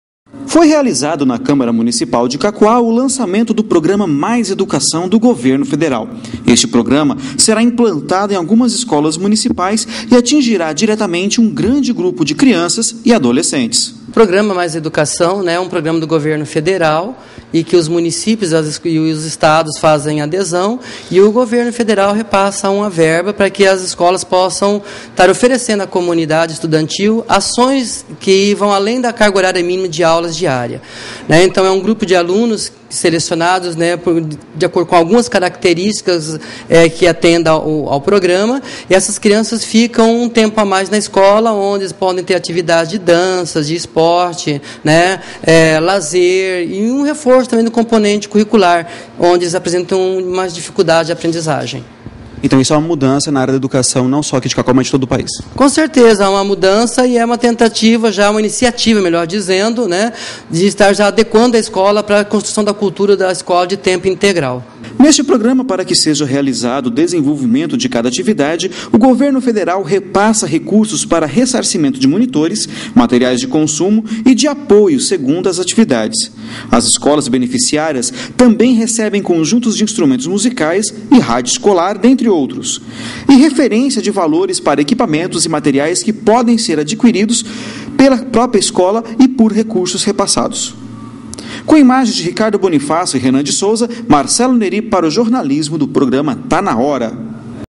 LANÇAMENTO PROJETO MAIS EDUCAÇÃO NAS ESCOLAS MUNICIPAIS DE CACOAL
Reportagem: Marcelo Nery
Imagens e edição: Ricardo Bonifácio